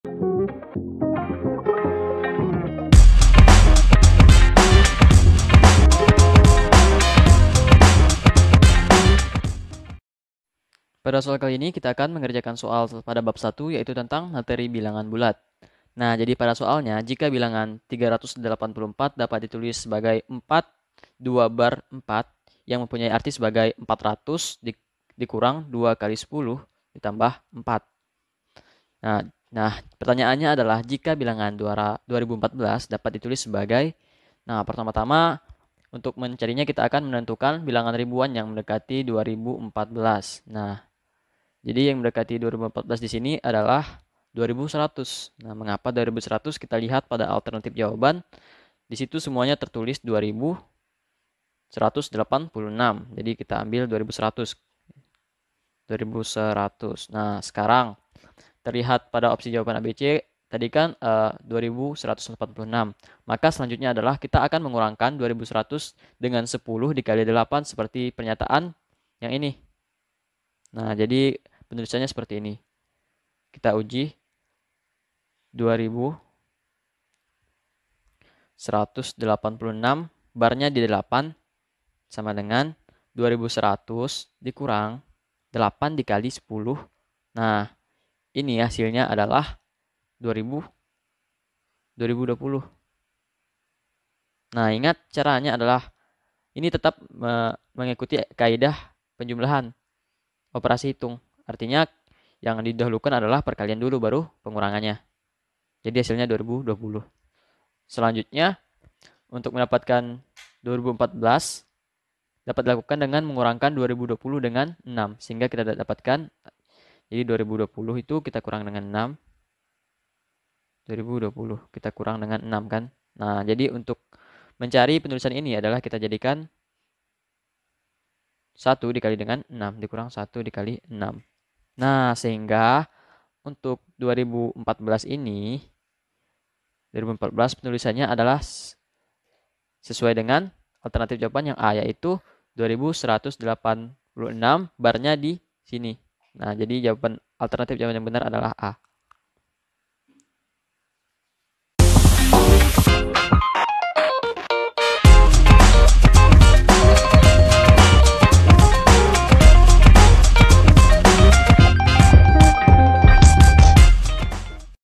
Bilangan 384 dapat ditulis sebagai ... | Bupena Matemati | Kelas 7 | 1 | Matematika
✅ Dalam video ini kita akan membahas: Jika bilangan 384 dapat ditulis sebagai 4bar{2}4  yang mempunyai arti sebagai  400-2×10+4 bilangan 2.014 dapat ditulis sebagai….
a. 21bar{86}
b. 2bar{1}86
c. 2bar{18}6
d. 218bar{6}
Tema:
Kelas: 7
Mata Pelajaran: Matematika
Bab: 1
Buku: Bupena Matematika Untuk SMP/MTS Kelas VII